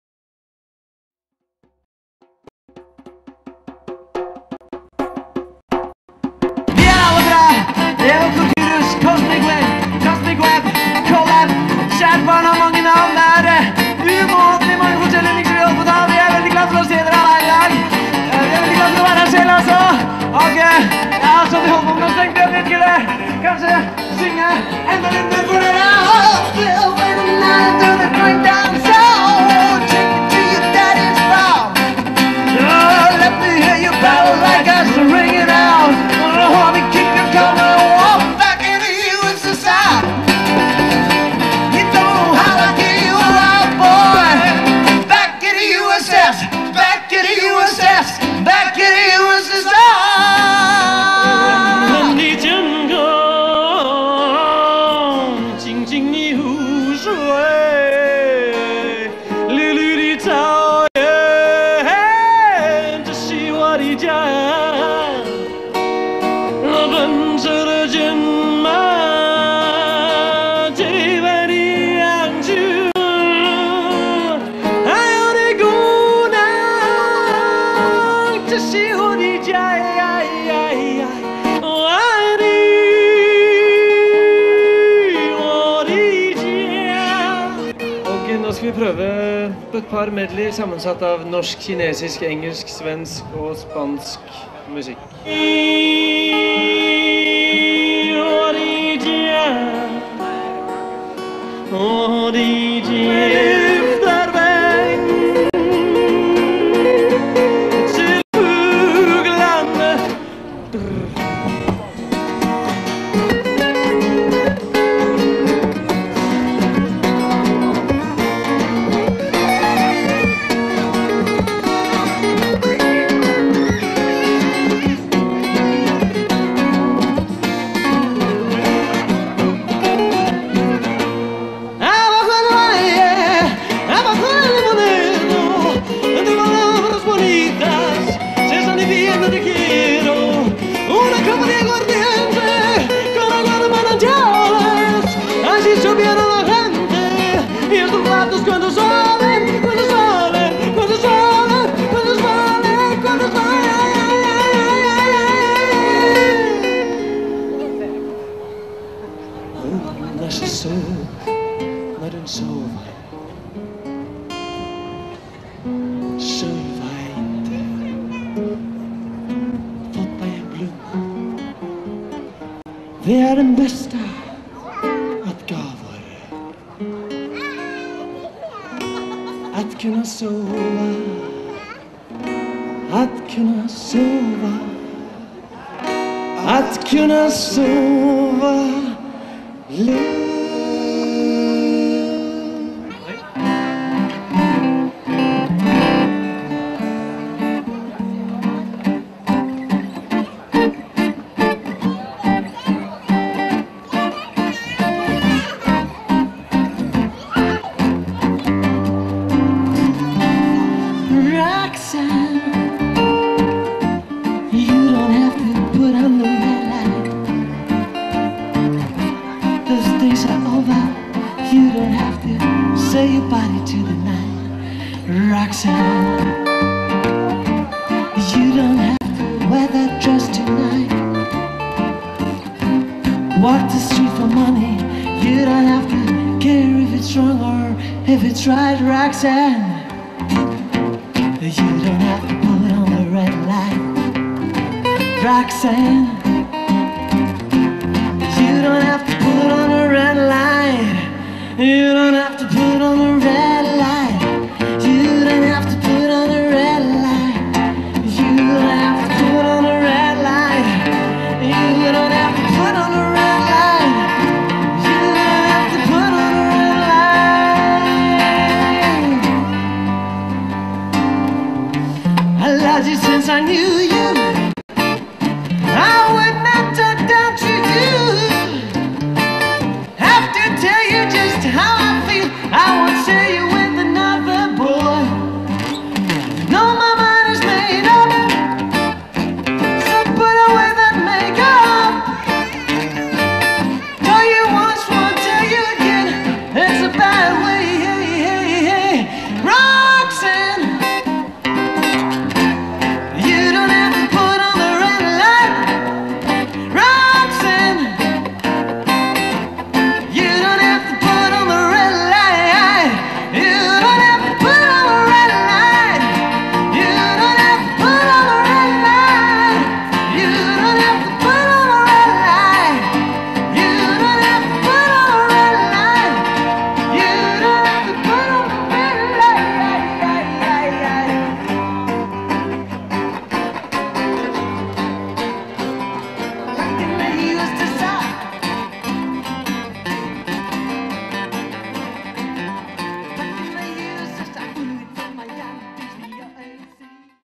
cosmic wind & eVo - musikkens dag - day of music -oslo 2008
5 språk medley, 5 language medley, Alx Kufot, Øystein Dahle Egset, Øyvind Aamot, Ivan Aleksander Reignaas, Wind, alias Øyvind Aamot and Dao Zhi Feng, 岛之风